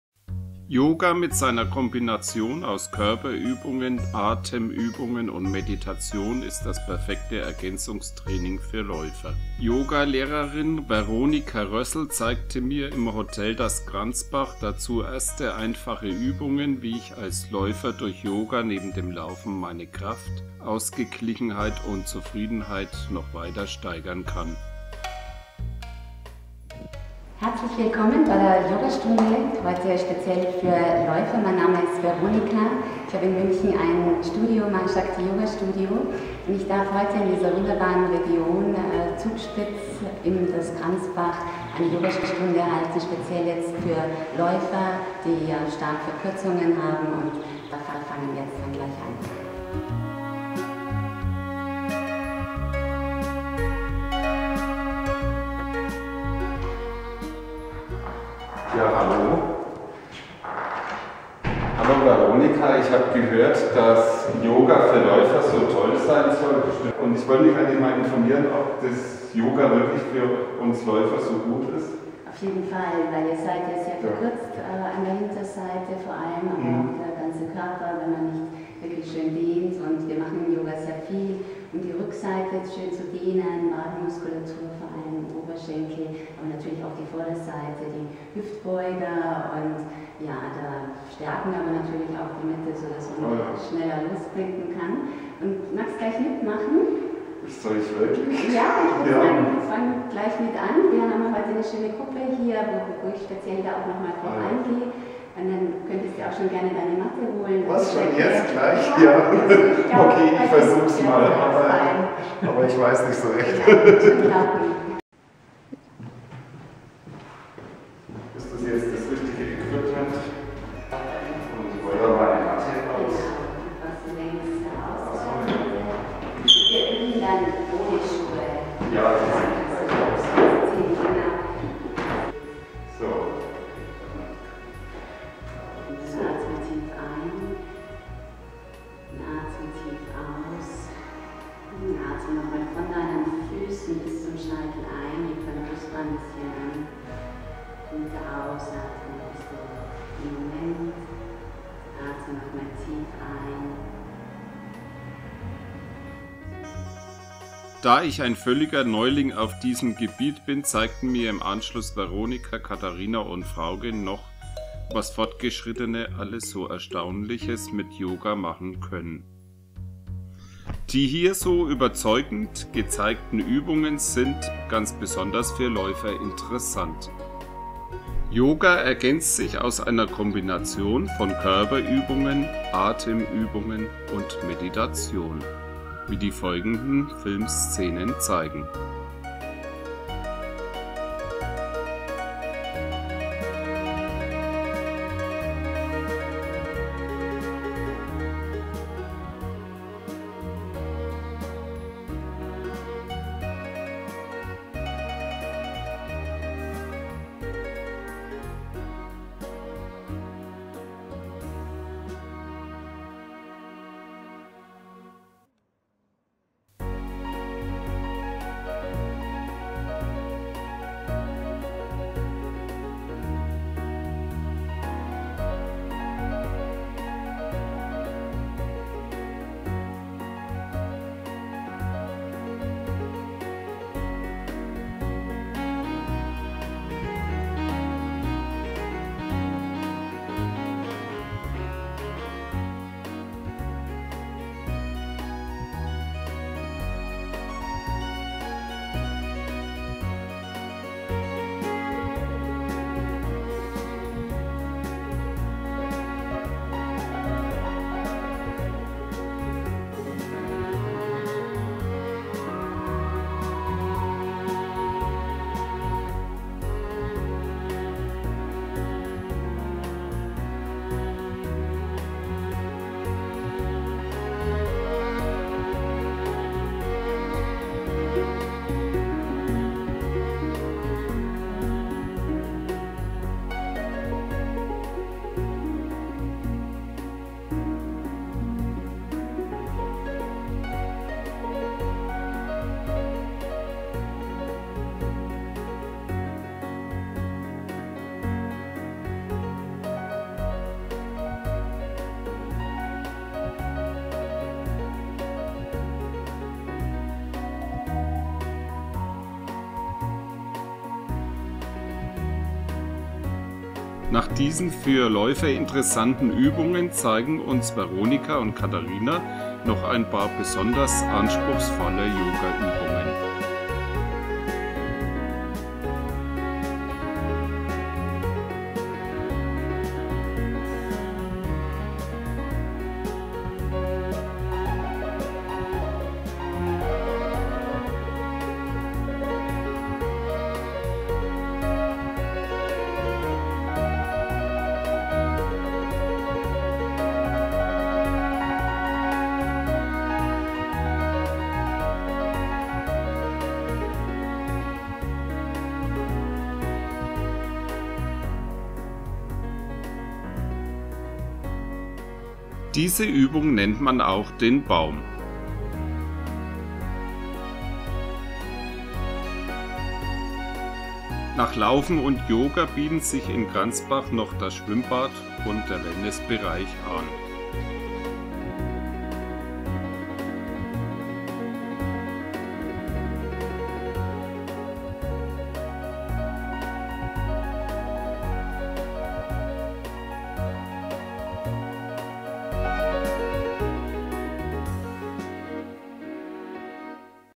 Yoga für Läufer in "Das Kranzbach"
Yoga mit seiner Kombination aus Körperübungen, Atemübungen und Meditation ist das perfekte Ergänzungstraining für Läufer.
Yoga Lehrerin Veronika Rössl zeigte mir im Hotel Das Kranzbach dazu erste einfache Übungen wie ich als Läufer durch Yoga neben dem Laufen meine Kraft, Ausgeglichenheit und Zufriedenheit noch weiter steigern kann.
Da ich ein völliger Neuling auf dem Gebiet bin, zeigten mir im Anschluss Veronika, Catharina und Frauke noch, was Fortgeschrittene alles so Erstaunliches mit Yoga  machen können!